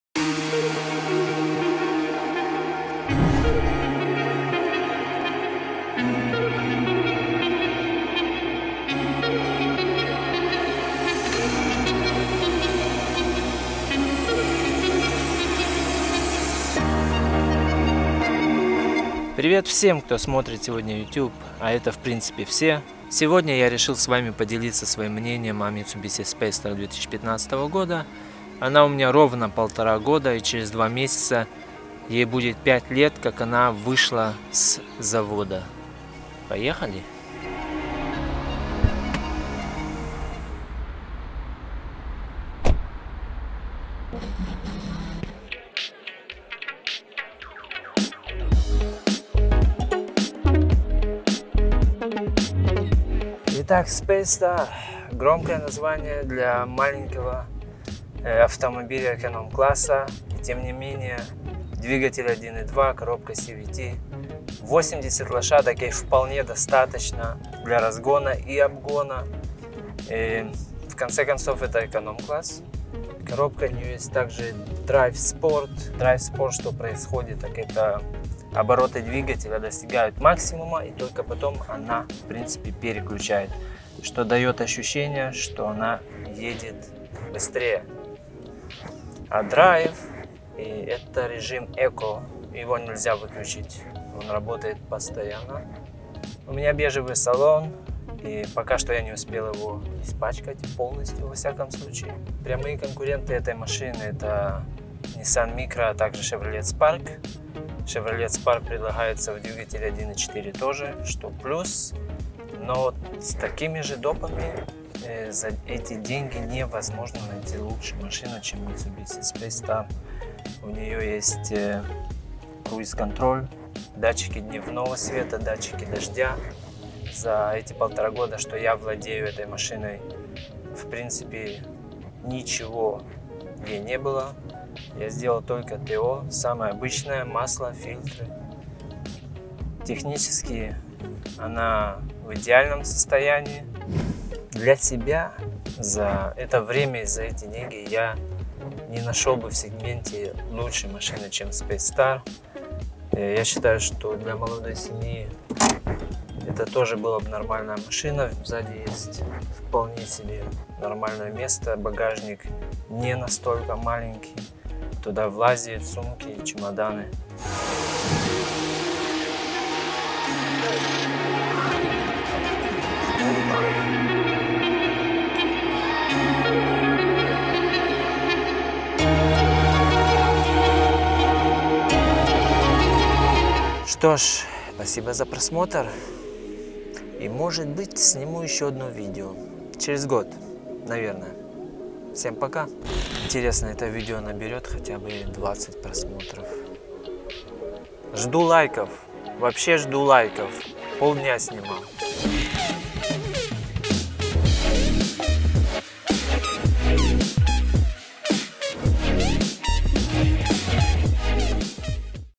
Обзор Митсубиси Спейс Стар 2015
Решил снова расказать о своей машине и заметил ,что минусов не упоменул походу я их незамечаю на фоне своей ласточки :)